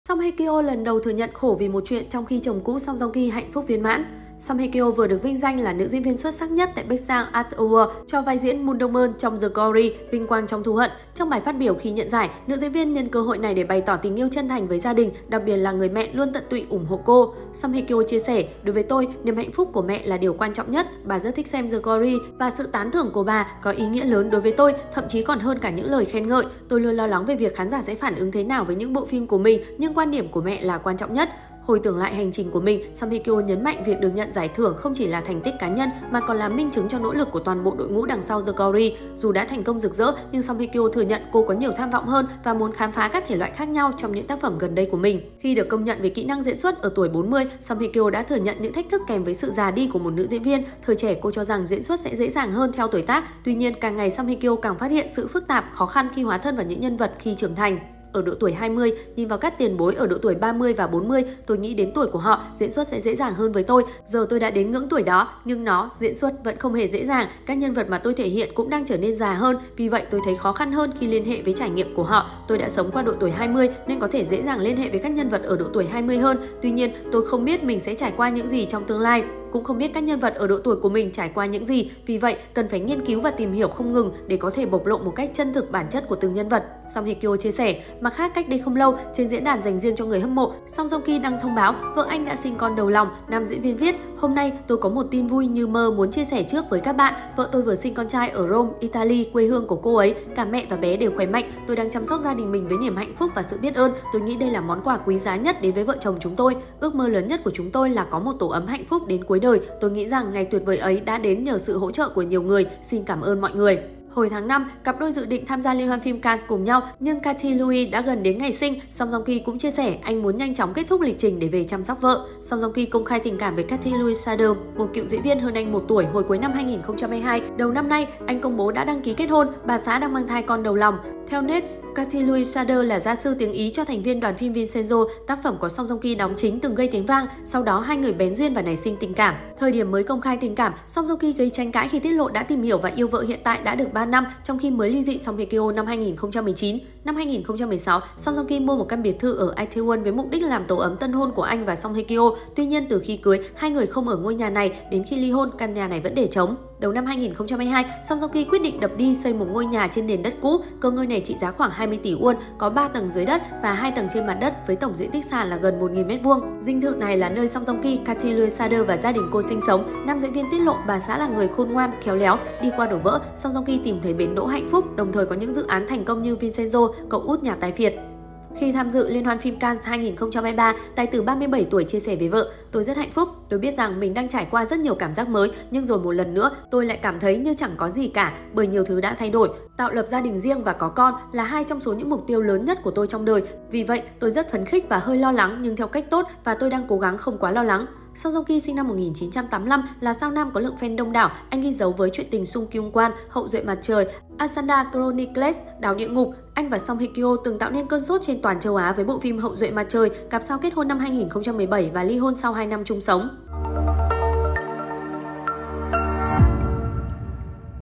Song Hye Kyo lần đầu thừa nhận khổ vì 1 chuyện trong khi chồng cũ Song Joong Ki hạnh phúc viên mãn
Song Hye Kyo vừa được vinh danh là Nữ diễn viên xuất sắc nhất tại Baeksang Arts Awards cho vai diễn Moon Dong Eun trong The Glory (Vinh Quang Trong Thù Hận). Trong bài phát biểu khi nhận giải, nữ diễn viên nhân cơ hội này để bày tỏ tình yêu chân thành với gia đình, đặc biệt là người mẹ luôn tận tụy ủng hộ cô.

Song Hye Kyo chia sẻ: "Đối với tôi, niềm hạnh phúc của mẹ là điều quan trọng nhất. Bà rất thích xem 'The Glory' và sự tán thưởng của bà có ý nghĩa lớn đối với tôi, thậm chí còn hơn cả những lời khen ngợi. Tôi luôn lo lắng về việc khán giả sẽ phản ứng thế nào với những bộ phim của mình nhưng quan điểm của mẹ là quan trọng nhất".